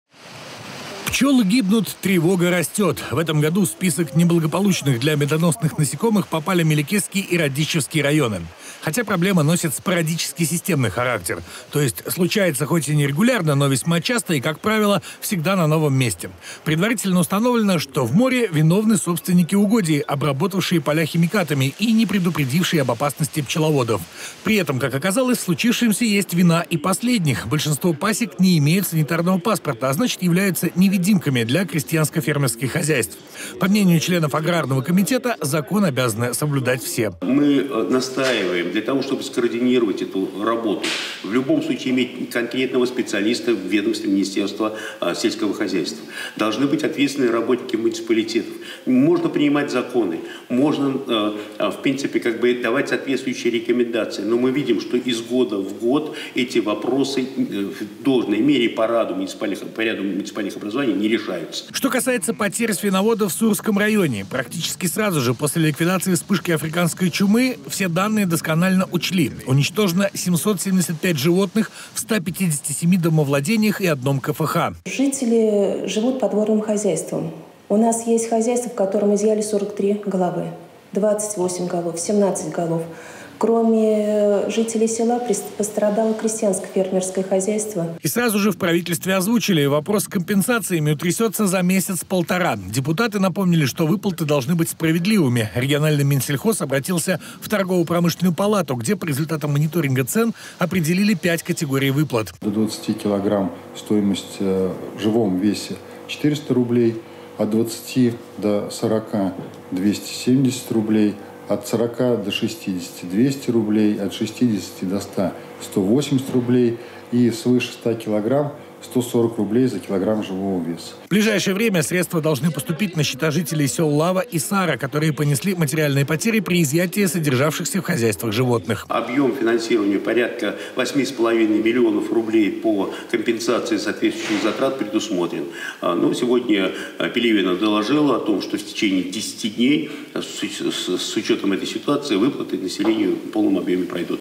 Пчёлы гибнут и тревога растёт, а также выплаты свиноводам
Аграрный комитет Законодательного собрания Ульяновской области обсудил массовую гибель пчёл в отдельных районах региона. Известно, что на 5 июня зафиксирована гибель 1149 пчёлосемей в 50 личных подсобных хозяйствах двух муниципалитетов. Кроме того, депутаты удовлетворились отчётами ответственных лиц о положении дел с выплатами свиноводам, пострадавшим от вспышки Африканской чумы свиней в Сурском районе. Агентство ветеринарии рассчитало, а правительство выделило необходимую компенсацию.

Когда средства поступят на счета владельцев потерпевших бедствие хозяйств?⠀⠀⠀⠀⠀